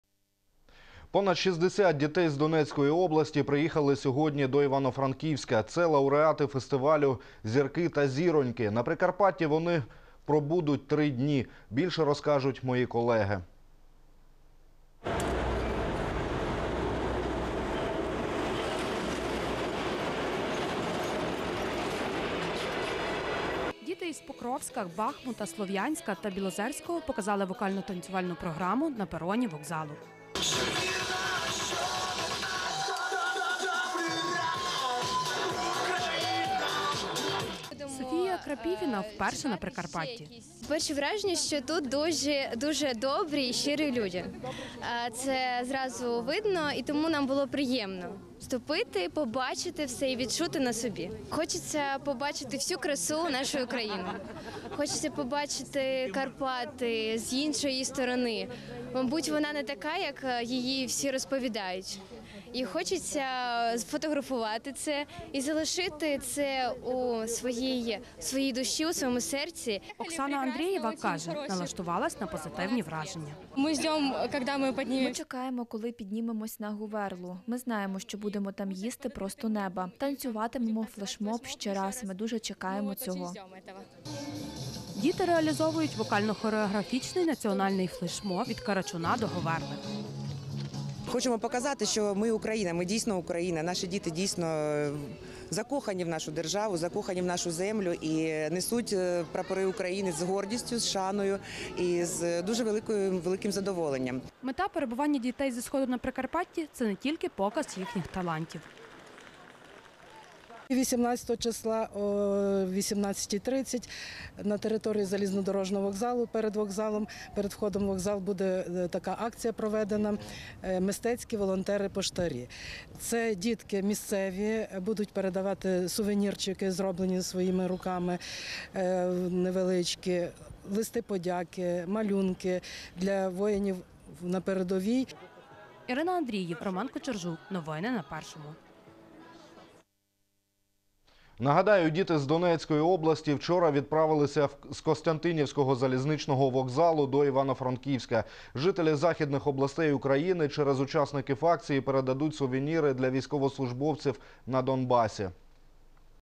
Діти з Донеччини приїхали на Прикарпаття
Понад 60 дітей з Донецької області приїхали сьогодні до Івано-Франківська. Це лауреати фестивалю “Зірки та зіроньки”. На Прикарпатті вони пробудуть три дні. Більш докладно про це дивіться у відео.